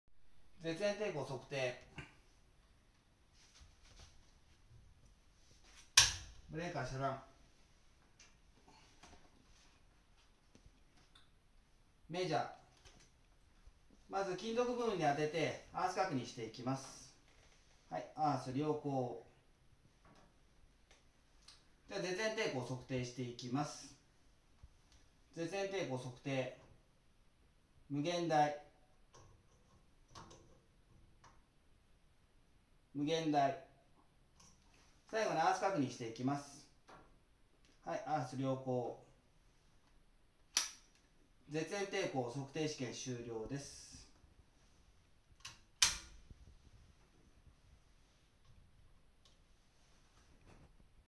消防設備点検における配線点検（絶縁抵抗測定）⑥ insulation resistance～小林消防設備 福岡県豊前市 全類消防設備士 第二種電気工事士 経営学修士～
全類消防設備士　第二種電気工事士の小林寮一が、消防設備点検で、配線を点検していきます。
配線の点検は、消防設備点検の中でも総合点検の点検項目で、年1回点検が行われます。
絶縁抵抗計（メガー）を使用して、絶縁抵抗を測定していきます。
絶縁抵抗測定は、回路に高電圧をかけ、抵抗値が高いことにより漏電がないことを確認するための試験です。